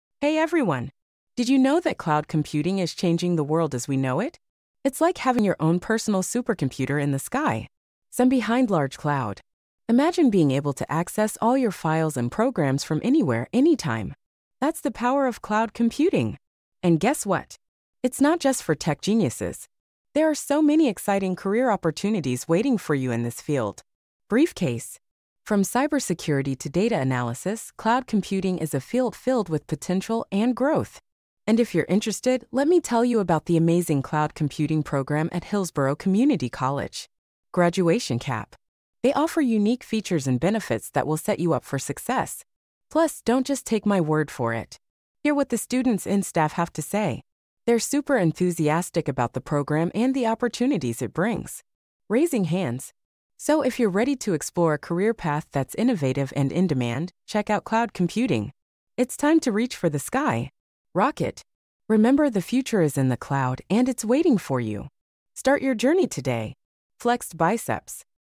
Hillsborough Community College_ Unlocking Cloud Computing A World of Possibilities for Students!
A short video about a high school event discussing the cloud computing program at Hillsborough Community College.